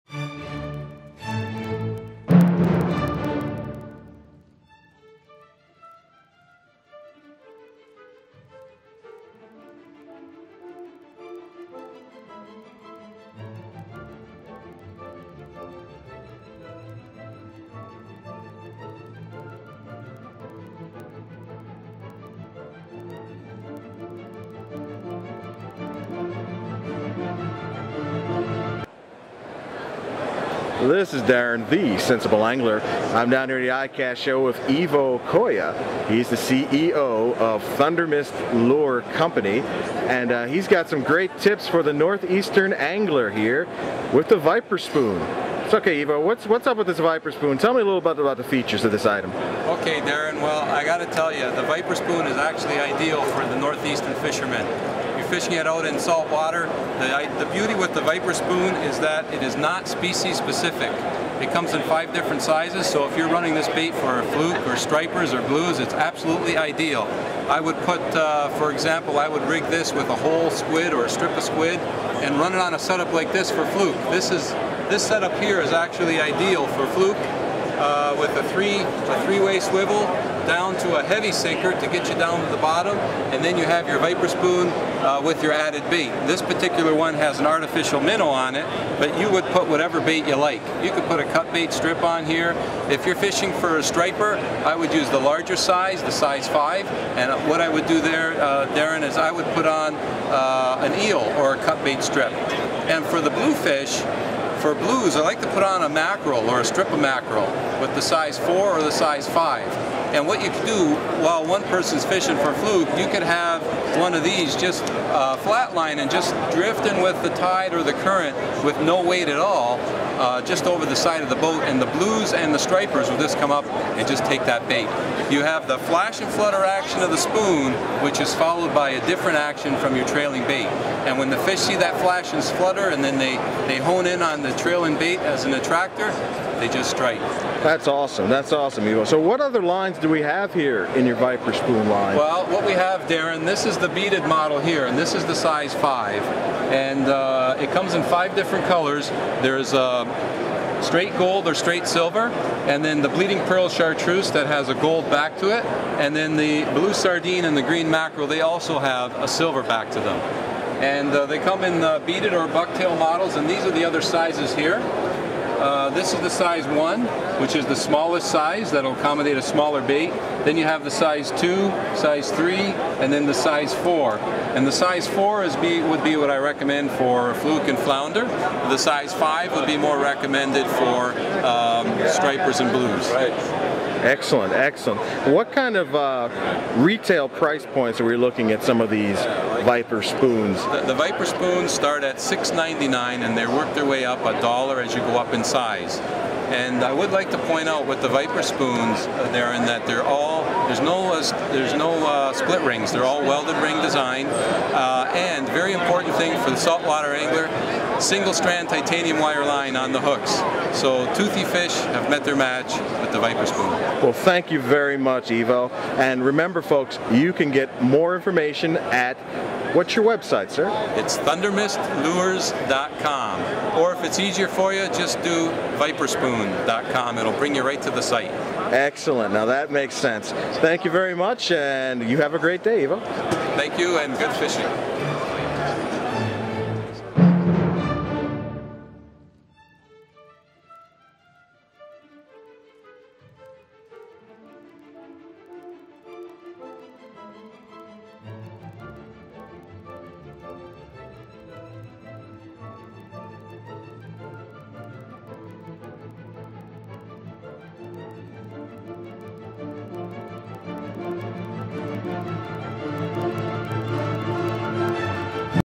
How to fish for Fluke, Bluefish & Stripers with Thundermist Lures
The Sensible Angler visits the Thundermist Lures Booth at ICAST and hears some great  tips on Northeast Saltwater fishing with the Viper Spoon.